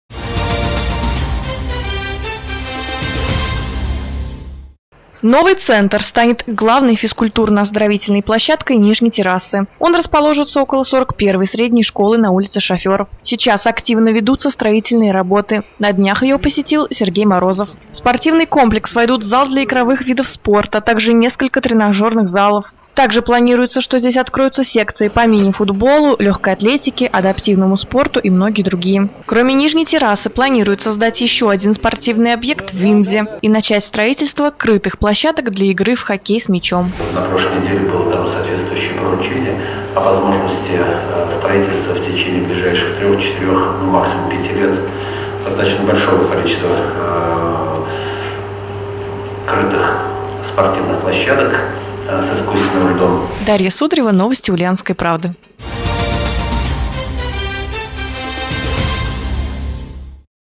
Новый ФОК на Нижней Террасе будет сдан осенью
Строительную площадку посетил глава региона.